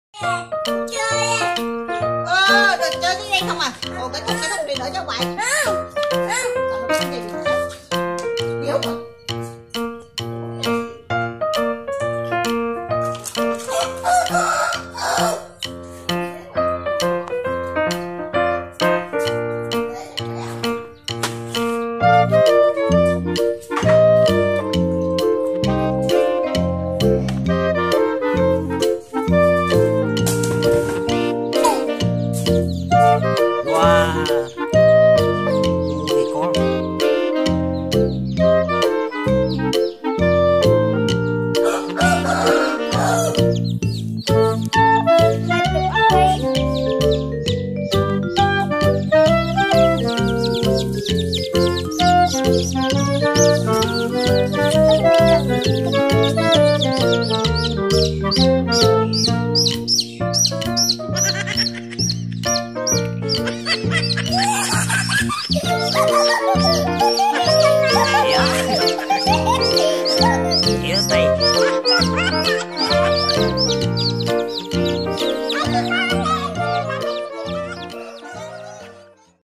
Trò chơi của Đạt bắt con gà 🐔 bỏ vào thùng
trò chơi của Đạt bắt con gà 🐔 bỏ vào thùng. giải trí lành mạnh. các bạn xem ủng hộ đăng ký kênh.© bản quyền thuộc về kênh Viet and Đat TV .